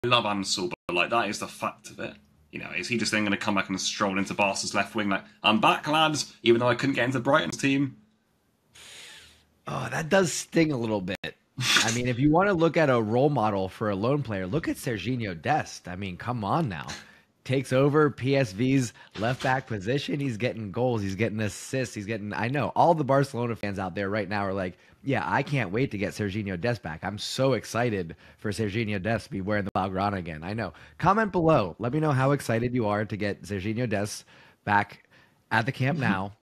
Sergino Dest RATED HIGHER Than Ansu Fati ...
Sergino Dest and Ansu Fati both out on loan from Barça. Should Ansu Fati take a lesson from Sergino Dest?